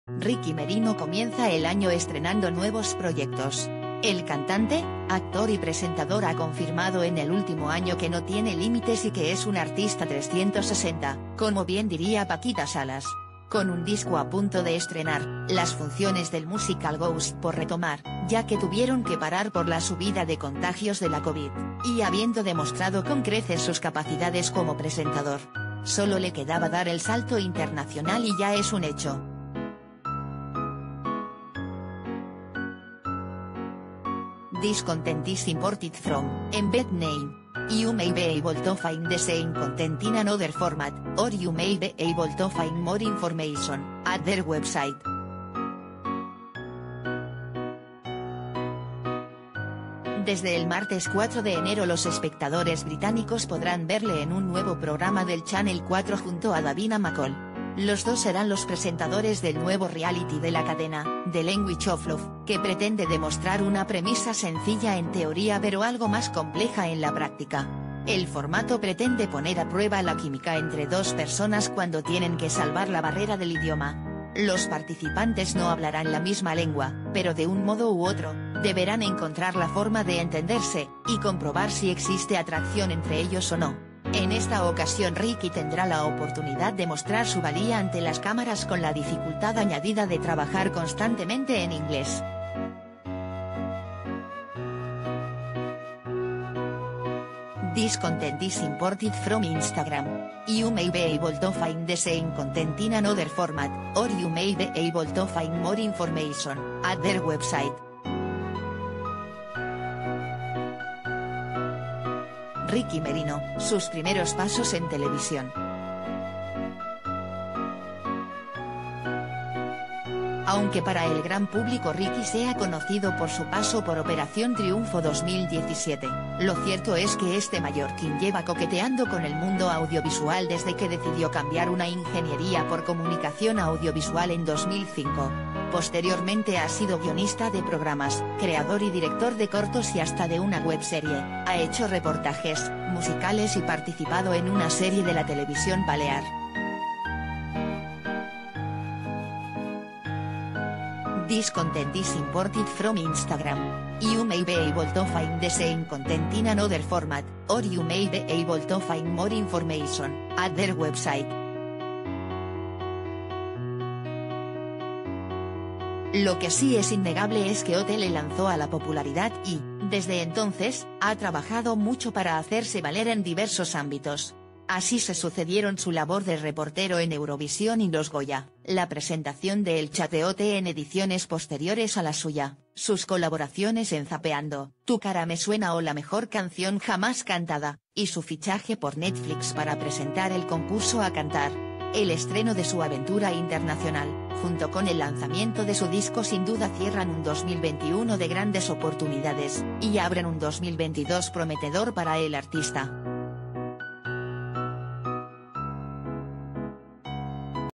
Ricky Merino da el salto al Channel 4 británico con 'The Language of Love'
Ricky Merino comienza el año estrenando nuevos proyectos. El cantante, actor y presentador ha confirmado en el último año que no tiene límites y que es un artista 360, como bien diría Paquita Salas. Con un disco a punto de estrenar, las funciones del musical Ghost por retomar, ya que tuvieron que parar por la subida de contagios de la covid, y habiendo demostrado con creces sus capacidades como presentador... solo le quedaba dar el salto internacional y ya es un hecho.
This content is imported